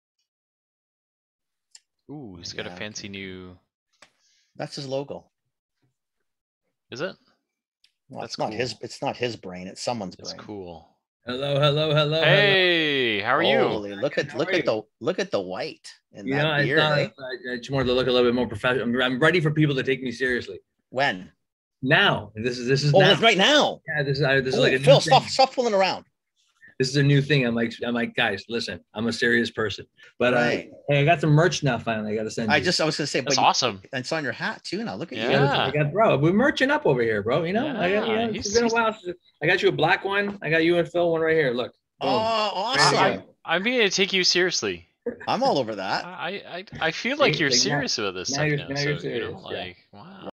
Steve Rad prepared to be taken seriously. New merch, new toys, virtual reality kicking up a notch
We stop fooling around. Steve Rad prepared to be taken seriously. Abacus Toys has new merch, new toys - virtual reality kicking up a notch. We also bend Kenny's brain with a virtual reality